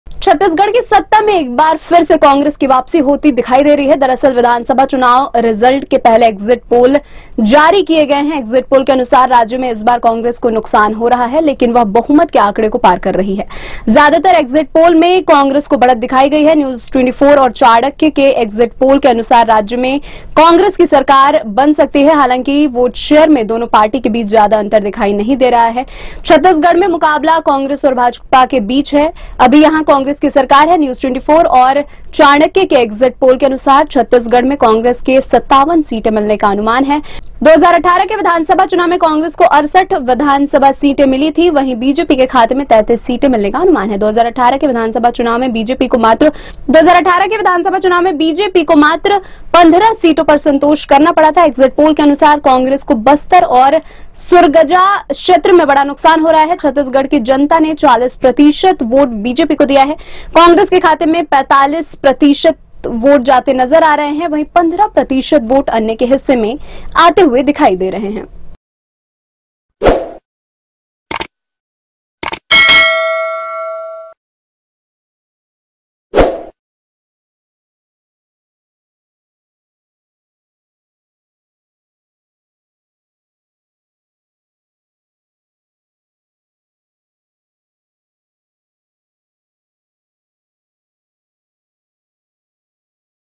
Chhattisgarh Exit Poll 2023 : एग्जिट पोल, छत्तीसगढ़ में फिर बन सकती है कांग्रेस की सरकार
छत्तीसगढ़ की सत्ता में एक बार फिर से कांग्रेस की वापसी होती दिख रही है। दरअसल, विधानसभा चुनाव रिजल्ट के पहले एग्जिट पोल जारी किए गए हैं। एग्जिट पोल के अनुसार, राज्य में इस बार कांग्रेस को नुकसान हो रहा है लेकिन वह बहुमत के आंकड़े को पार कर रही है। ज्यादातर एग्जिट पोल में कांग्रेस को बढ़त दिखाई गई है। न्यूज-24 और चाणक्य के एग्जिट पोल के अनुसार, राज्य में कांग्रेस की सरकार बन सकती है। हालांकि वोट शेयर में दोनों पार्टी के बीच ज्यादा अंतर नहीं दिखाया गया।  छत्तीसगढ़ में मुकाबला कांग्रेस और भाजपा के बीच है। अभी यहां कांग्रेस की सरकार है।